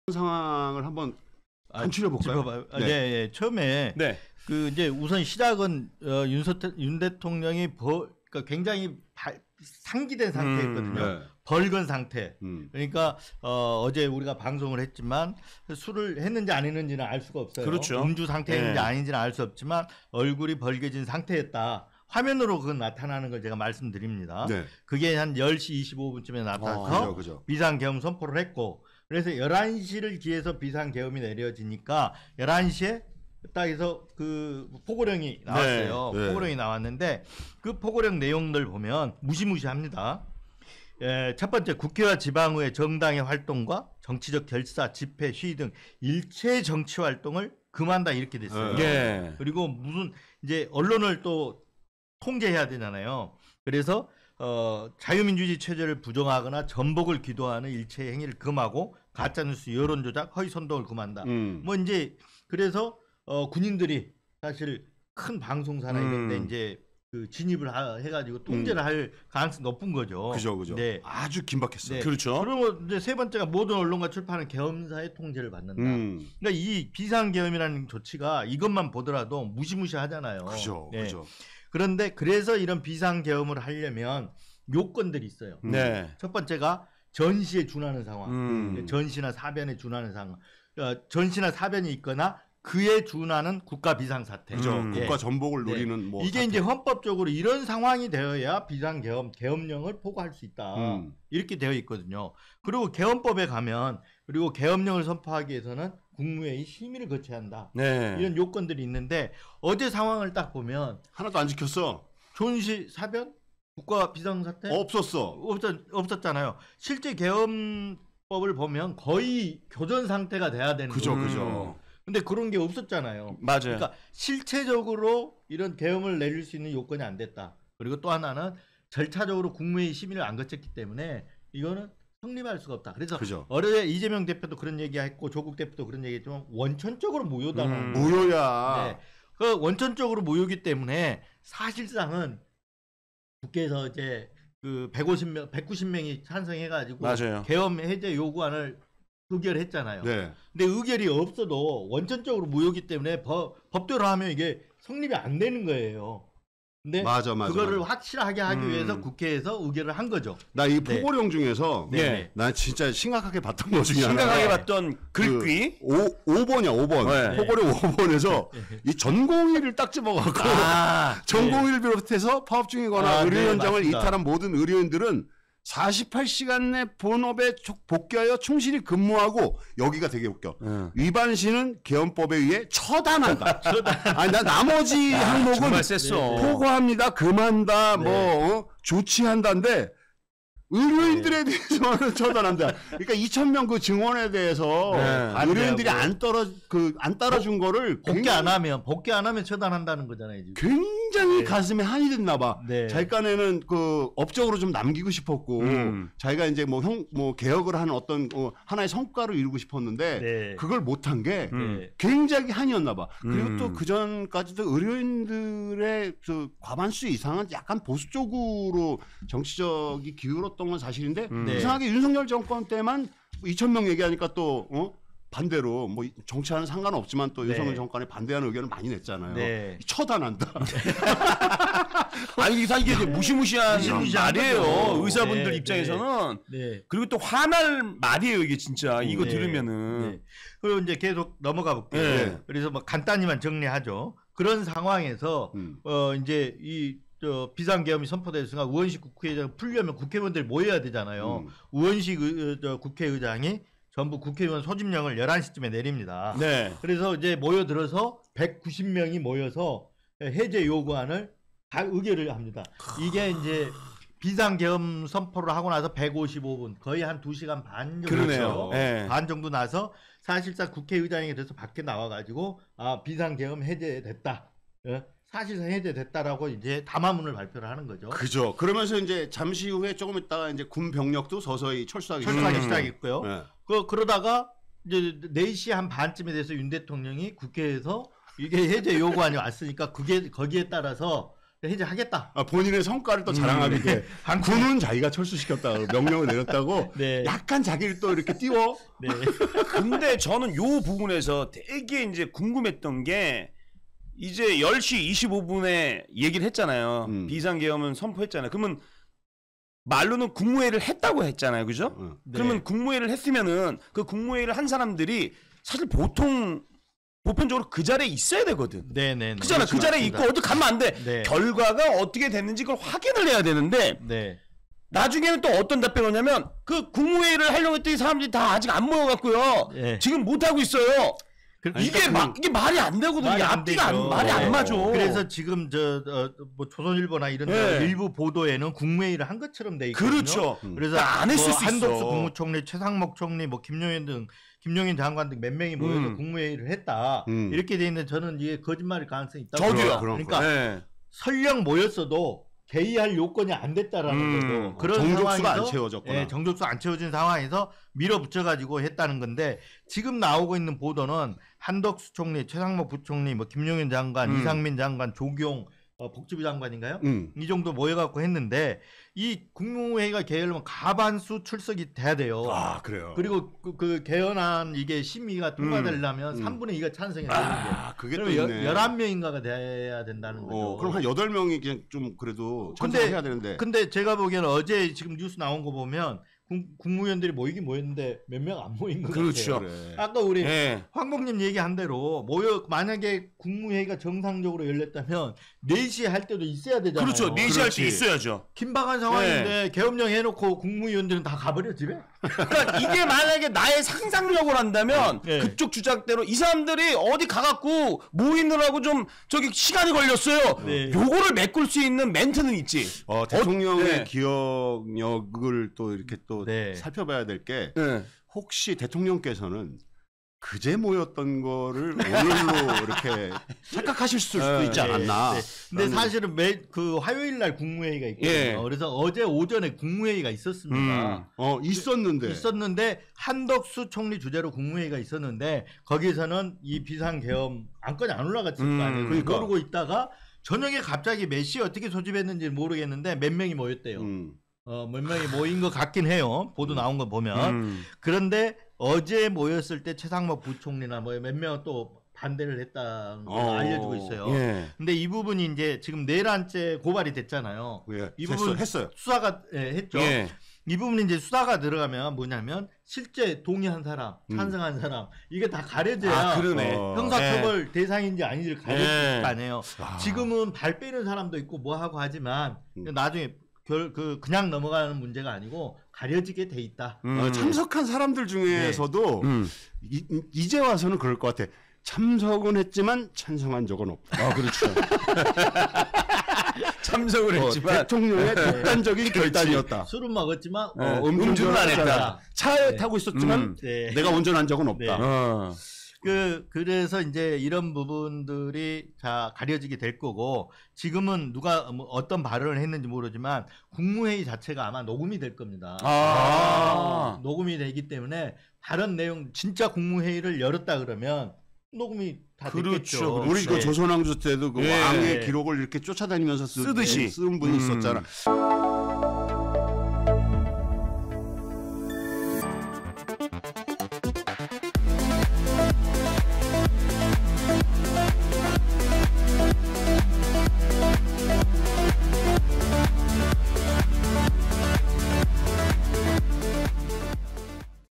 이진동 기자의 날카로운 지적! 尹 계엄령 선포가 원천무효인 이유 (24/12/04)
📌뉴스버스 ARS 후원▶▶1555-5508
📌뉴스버스 후원계좌▶▶신한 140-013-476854 / (주)위더미디어

편집 : 김준영